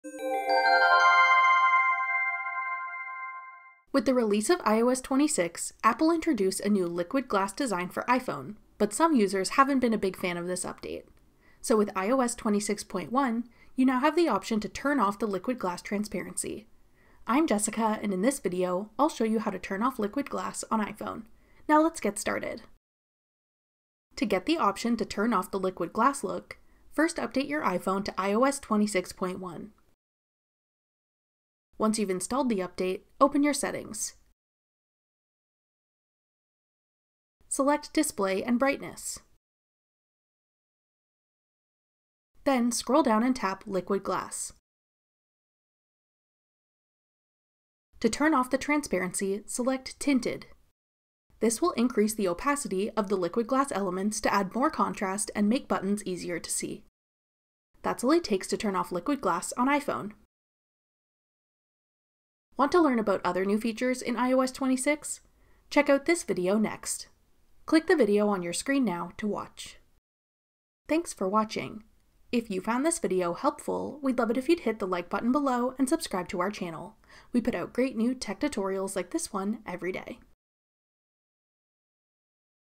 How to Turn Off Liquid Glass on iPhone in iOS 26 Update (New Feature)
With the release of iOS 26, Apple introduced a new Liquid Glass design for iPhone, but some users haven’t been a big fan of this update. So with iOS 26.1, you now have the option to turn off the Liquid Glass transparency. Watch this video to learn how to turn off Liquid Glass on iPhone.

To get the option to turn off the Liquid Glass look, first update your iPhone to iOS 26.1. Check out this video to learn how to update your iPhone

Once you’ve installed the update, open your Settings. Select Display and Brightness. Then scroll down and tap Liquid Glass. To turn off the transparency, select Tinted. This will increase the opacity of liquid glass elements to add more contrast and make buttons easier to see.